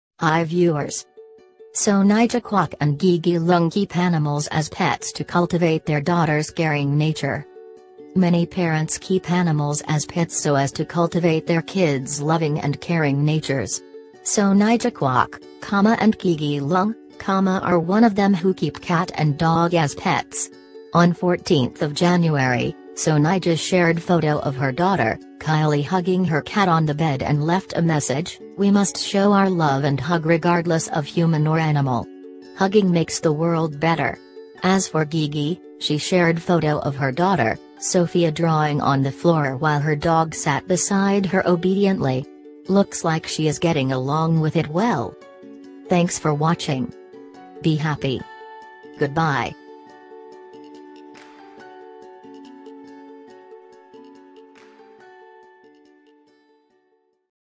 Sonija Kwok and Gigi Leung keep animals as pets to cultivate their daughter's caring nature
TODAY I AM BRINGING YOU A NEW VIDEO AND UPLOAD A VIDEO ON YOUTUBE FOR YOUR ENJOYMENT AND INFORMATION.

Source of this report: Source: Ming Pao (HK)
Translated by: tvfan @ Asian E-News Portal